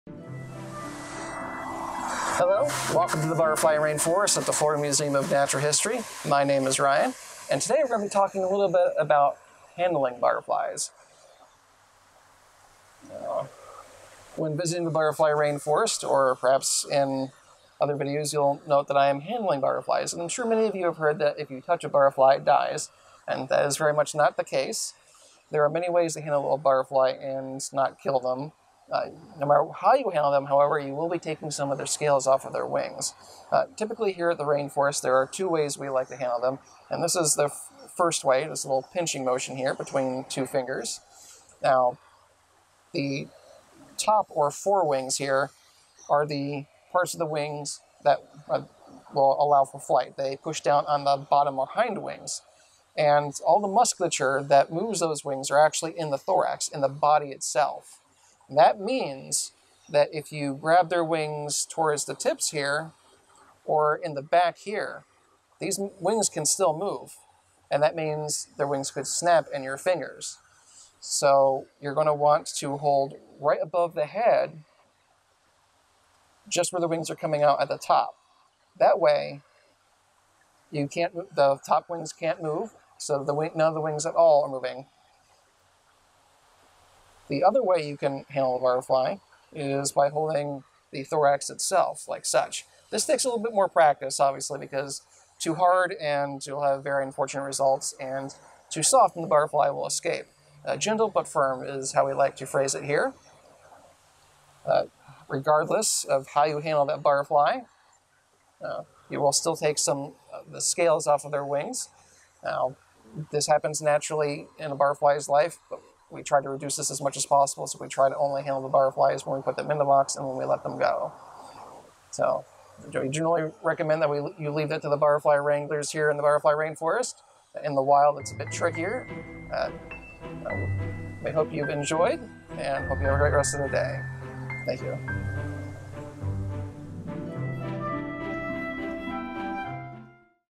Butterfly Rainforest Moment: Handling butterflies
Spend a moment in our Butterfly Rainforest with Ryan talking about the proper way to handle butterflies. Although there are recommended ways to safely hold a butterfly, any way you do it, you're taking scales off their wings.

We only handle butterflies on a few occasions, and we ask our visitors to refrain from touching butterflies in the Butterfly Rainforest.